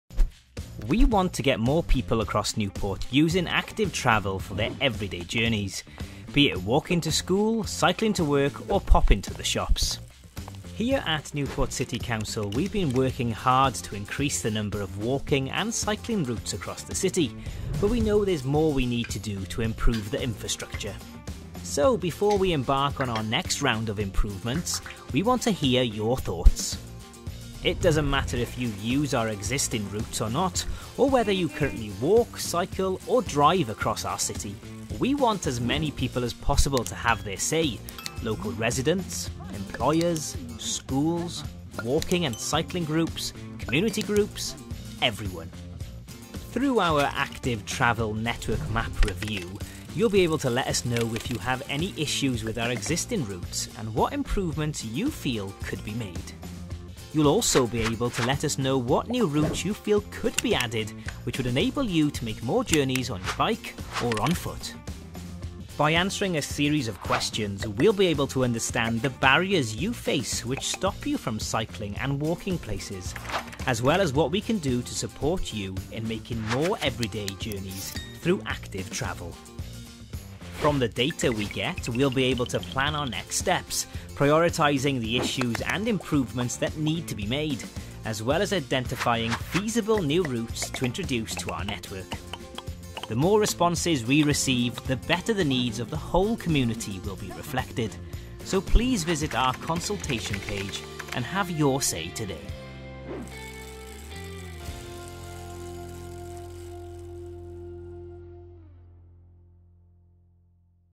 jamjar x Newport City Council Active Travel Consultation Animation
How do you engage the public in a consultation? Employ us to create an engaging animation, of course! Here's our latest animation for @newportcitycouncil to promote its Active Travel consultation.